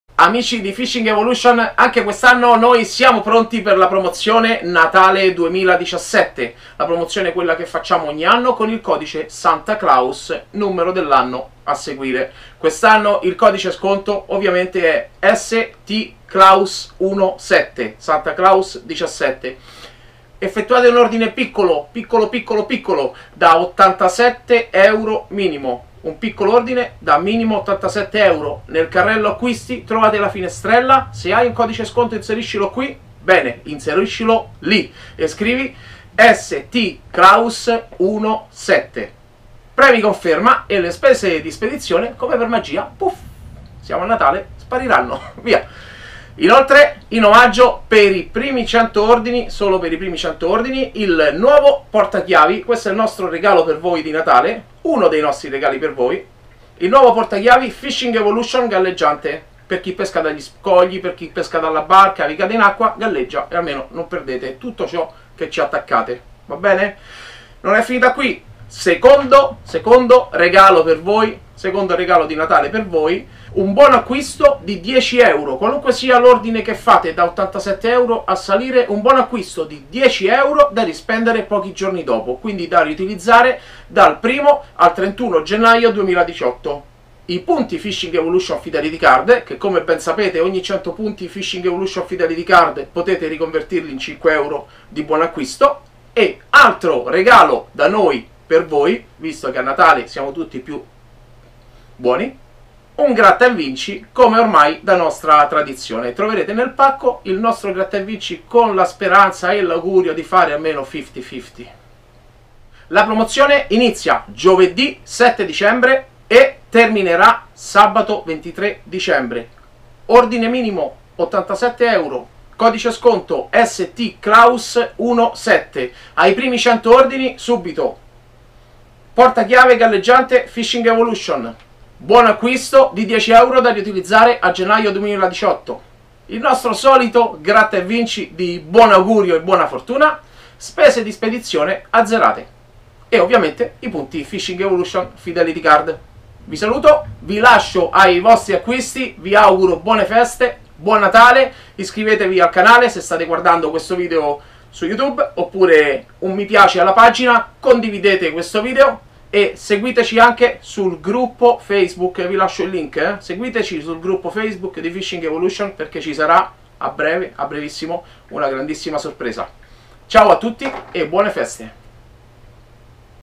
Promozione Natale 2017: Codice sconto STCLAUS17
Promo Natale 2017. Codice sconto STCLAUS17.
Da Giovedi 7 a Sabato 23 Dicembre, effettua un minimo ordine di 87 euro, inserisci il Codice STCLAUS17 e subito per te:
1- Spedizioni Gratuite
2- Buono Acquisto da 10 Euro da riutilizzare a Gennaio 2018
3- il Porta Chiavi Fishing Evolution Float
4- un Gratta e Vinci di Buon Augurio
5- i PUnti FE Fidelity Card.
Attenzione: Buono Acquisto, Porta Chiavi e Gratta e Vinci solo nei primi 100 ORDINI!!!
Affrettatevi!